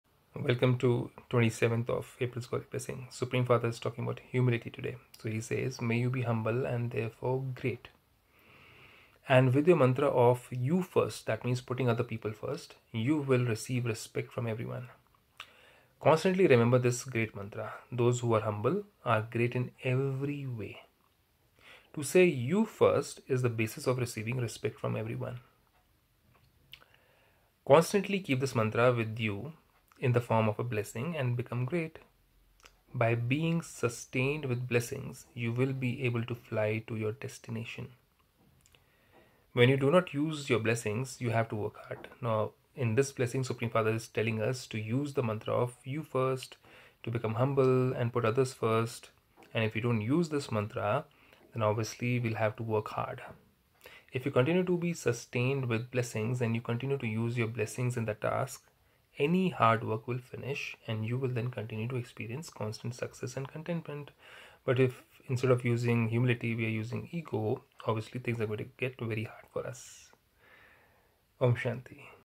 Be humble & therefore great & with your mantra of “You first” you will receive respect from everyone
Day 814 May you be humble and therefore great and, with your mantra of “You first”, you will receive respect from everyone